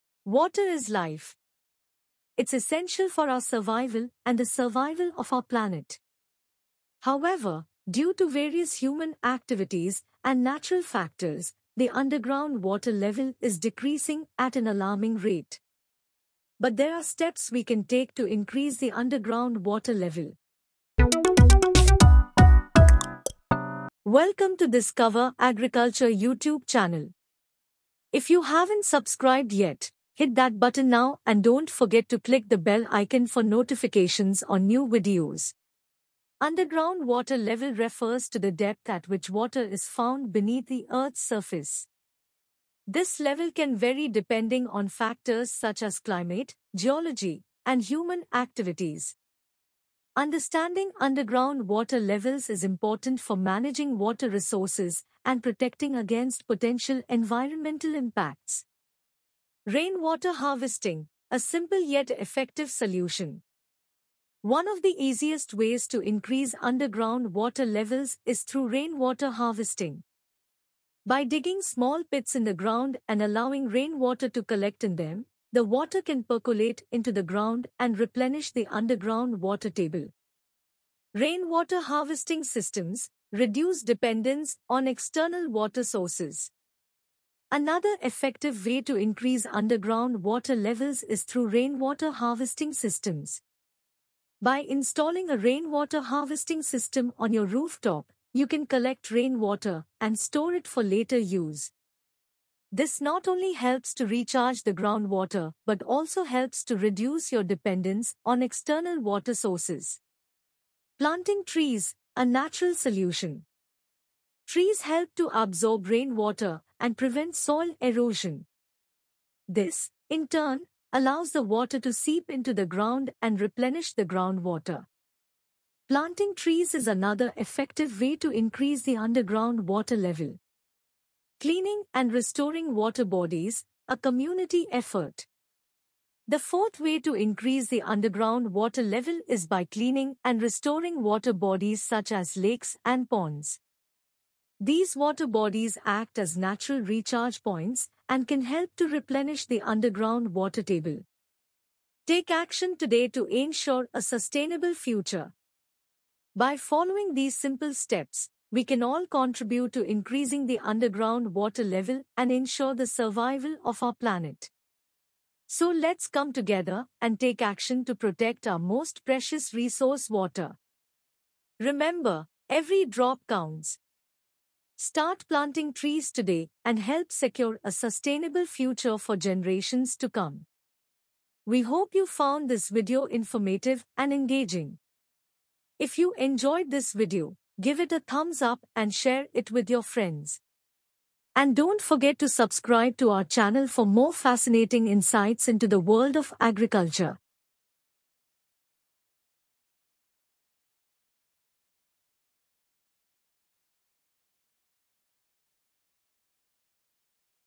How to increase Groundwater Level? Groundwater Recharging Methods
In this video, we will discuss various methods to increase the groundwater level in your area. Groundwater is a vital resource that is essential for the survival of all living beings. However, due to various factors such as over-extraction and climate change, the groundwater level is depleting at an alarming rate. Our aim is to help you understand the importance of groundwater and the ways in which you can contribute towards its conservation.

In this video, we will cover the following topics:

Understanding the importance of groundwater
Factors affecting groundwater level
Different methods to recharge groundwater
Benefits of groundwater recharging
At Discover Agriculture, we are committed to spreading awareness about environmental conservation and sustainable living. Watch this video to learn more about groundwater recharging and how you can make a difference in your community.

0:00 Introduction
0:36 Underground water
1:00 Rainwater Harvesting: A Simple Yet Effective Solution
1:22 Rainwater Harvesting Systems: Reduce Dependence on External Water Sources
1:51 Planting Trees: A Natural Solution
2:12 Cleaning and Restoring Water Bodies: A Community Effort
2:45 Conclusion
3:01 Bottom line